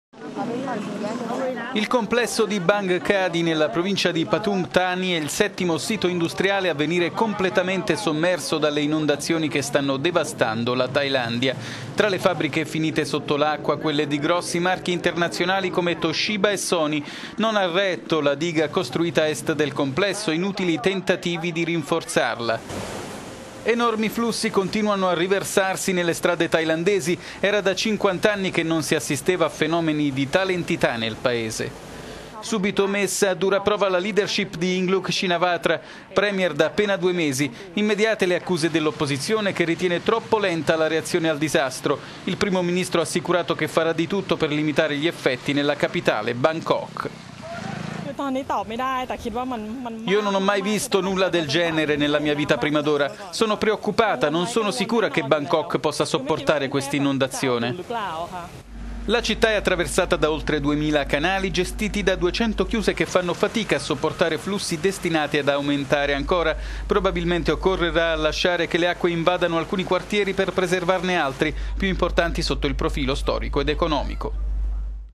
Thailandia, le inondazioni sommergono il complesso di Bang Kadi
Il complesso di Bang Kadi, nella provincia di Pathum Tani, è il settimo sito industriale a venire completamente sommerso dalle inondazioni che stanno devastando la Thailandia.

Tra le fabbriche finite sotto l'acqua quelle di grossi marchi internazionali come Toshiba e Sony. Non ha retto la diga costruita a est del complesso, inutili i tentativi di rinforzarla.

Enormi flussi continuano a riversarsi nelle strade thailandesi. Era da 50 anni che non si assisteva a fenomeni di tale entità nel Paese.